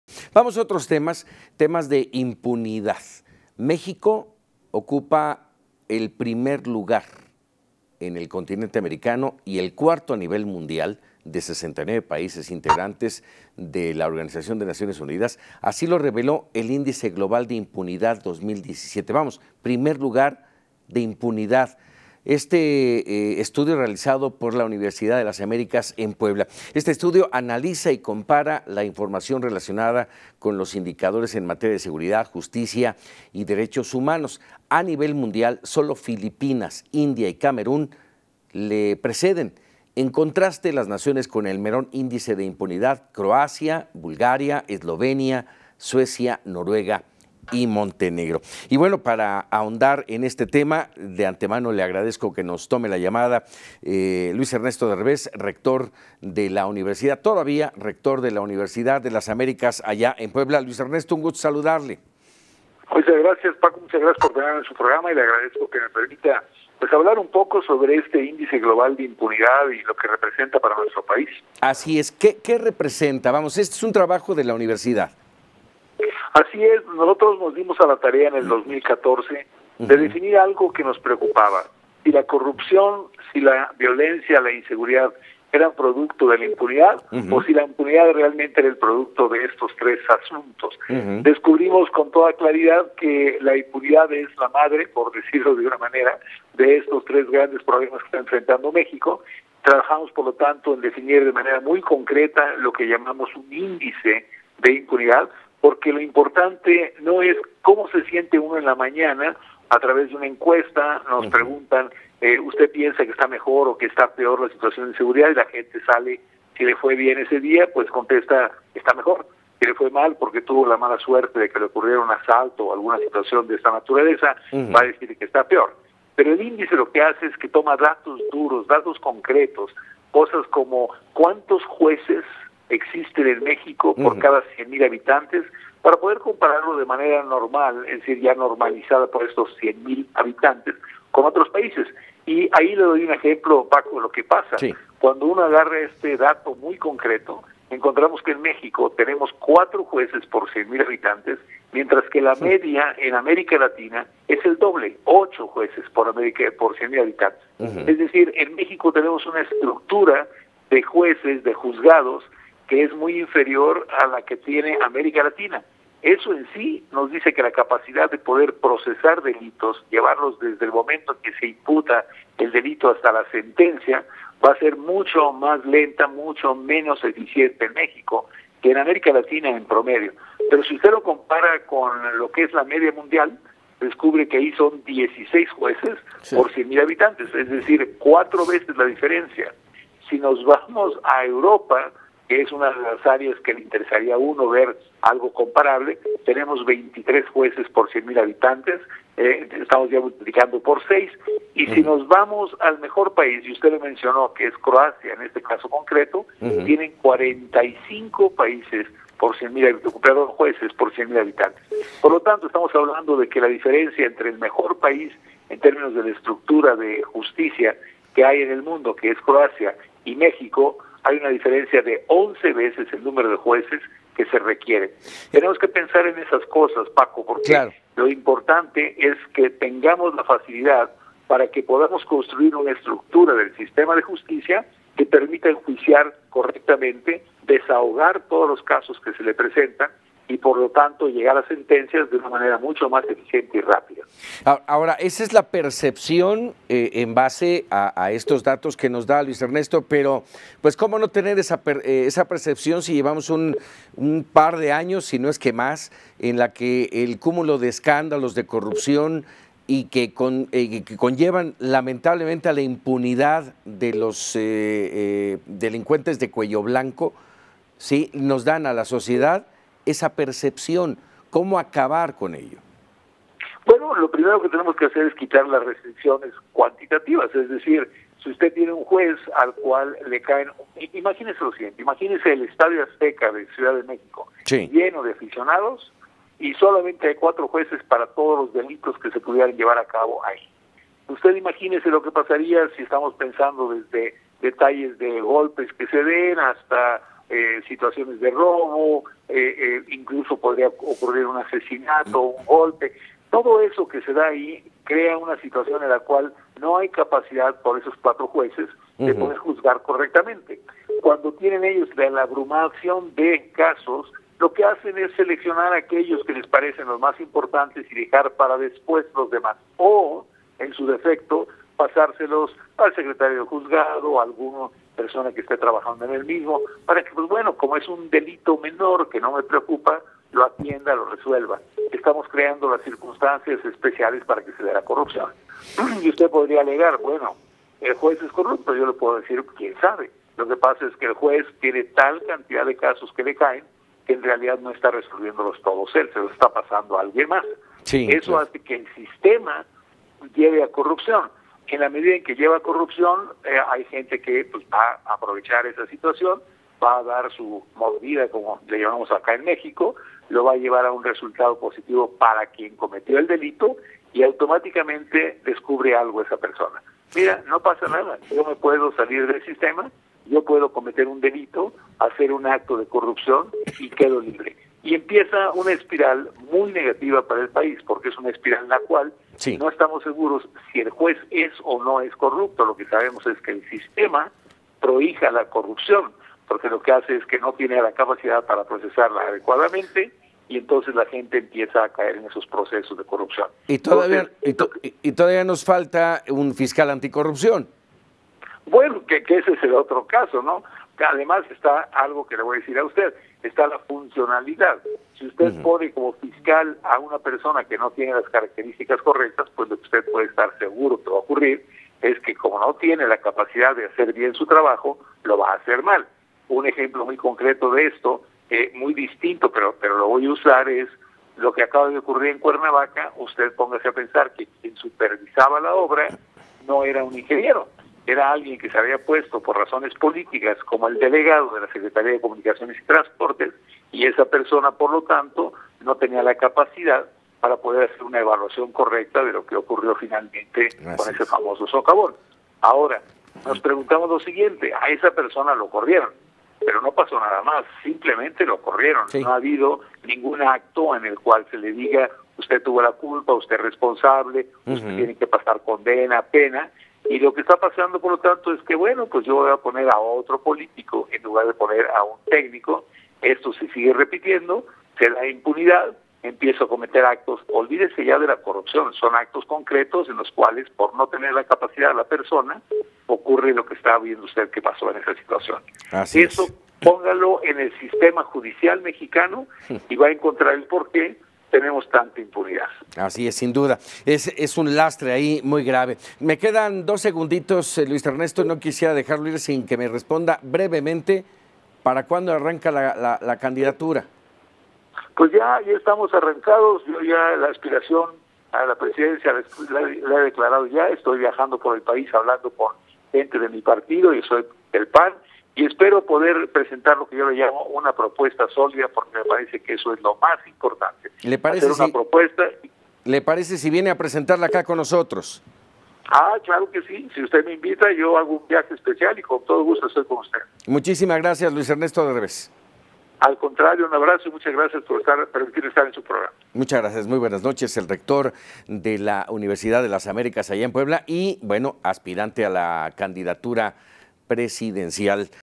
Entrevista a Luis Ernesto Derbez rector de la UDLAP sobre impunidad en México
México ocupa el primer lugar en impunidad a nivel continental, el rector de la UDLA, Luis Ernesto Derbez platica sobre el tema.